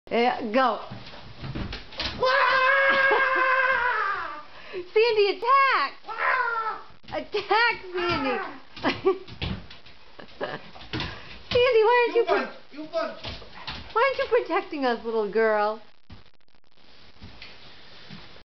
Sandy takes on a ninja
Sandy protects her home from a crazed ninja.